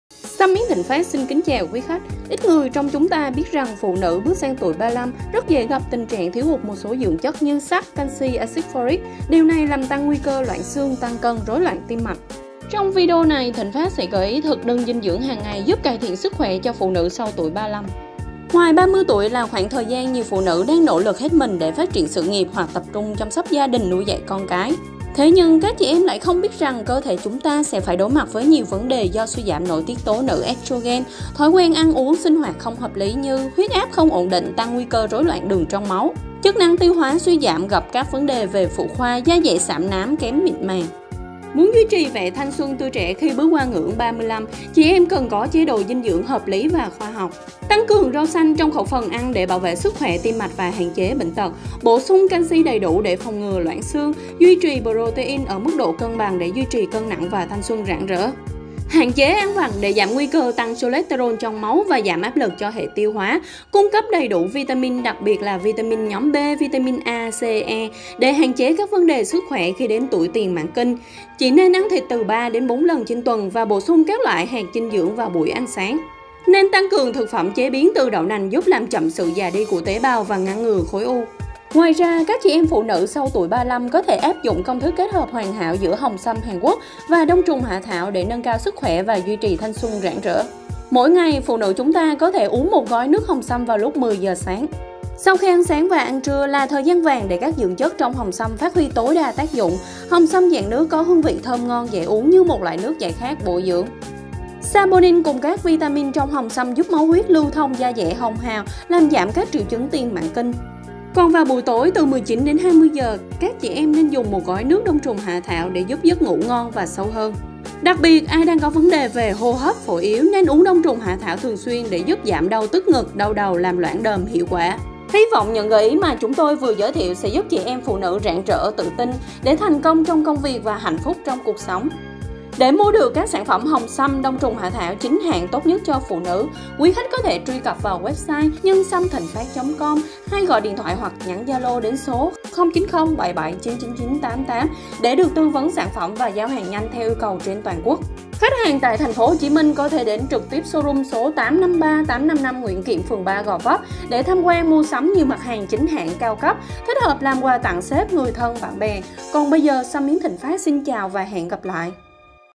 Thực đơn dinh dưỡng cho phụ nữ sau 35 tuổi
🌹 Khi bước sang tuổi 35, nhiều phụ nữ thường gặp thiếu hụt một số dưỡng chất như sắt, calci, acid folic, làm tăng nguy cơ mắc một số vấn đề như tăng cân, loãng xương, cơ bắp yếu…

👉 Trong video này, Thịnh Phát xin gợi ý đến quý khách thực đơn dinh dưỡng hằng ngày rất tốt cho phụ nữ sau tuổi 35 mà các chị em có thể tham khảo.

👉 Đặt hàng các sản phẩm chăm sóc sức khỏe tốt nhất cho nữ giới tại
🔎 MỌI GÓP Ý XIN GỬI VỀ:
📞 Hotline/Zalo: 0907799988